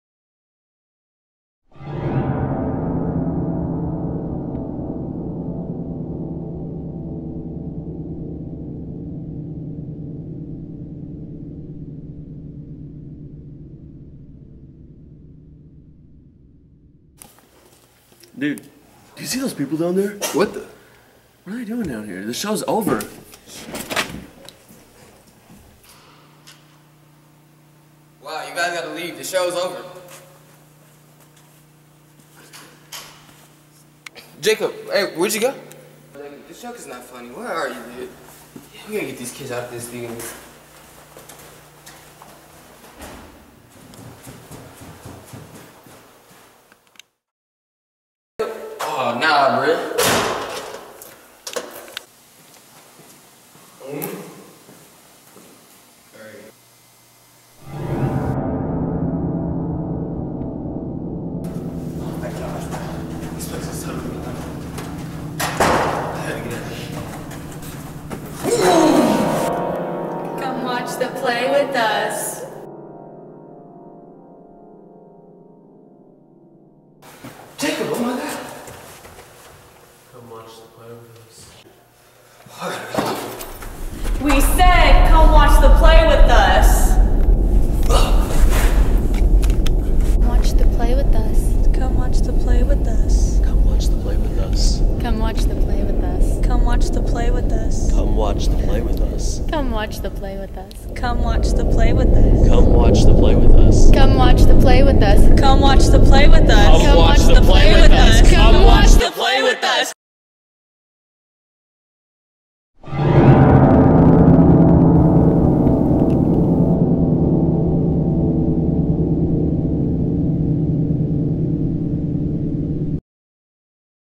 Watch the Play With Us
AV1- Jacob, Reyann, Blake and Emily's Horror Film "Watch the Play With Us"